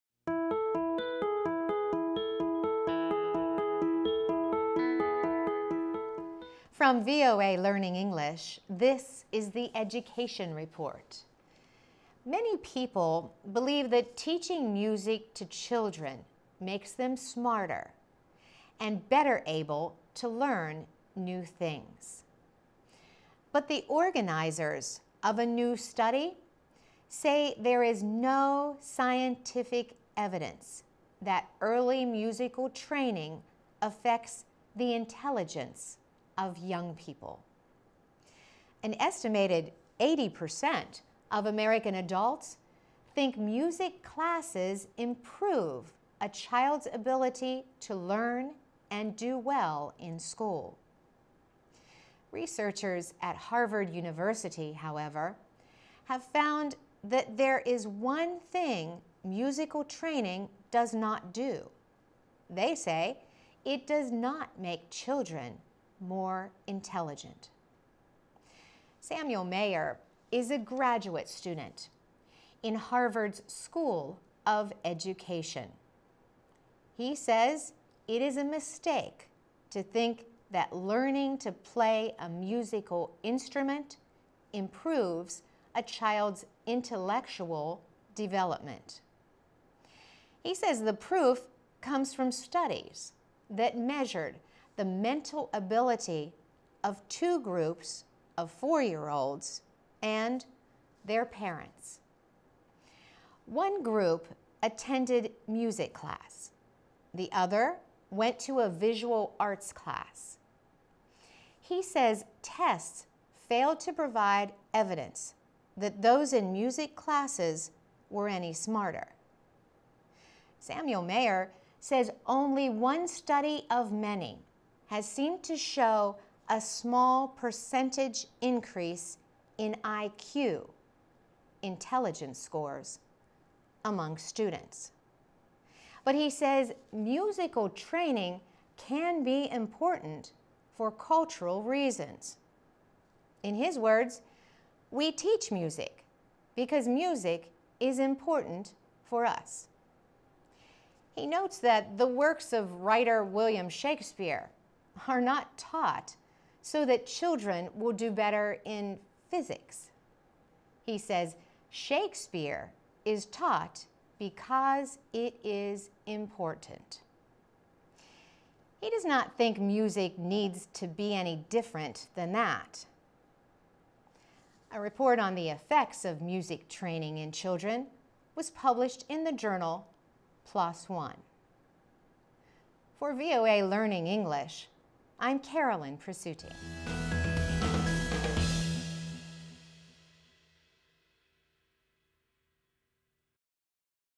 Research Say Ideas About Music and Intelligence Are...
Organizers of a new study say there is no scientific evidence that early musical training affects the intelligence of young people.